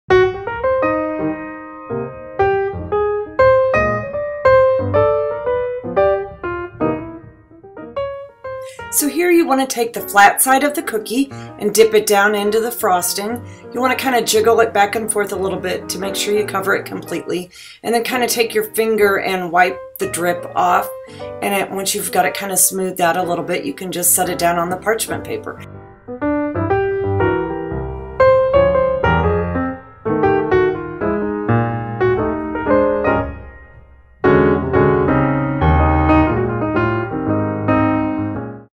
How to Frost Berger Cookies
Copycat recipe for Baltimore's iconic Berger Cookies- this video shows you how to frost these soft vanilla cookies with a rich fudge like frosting.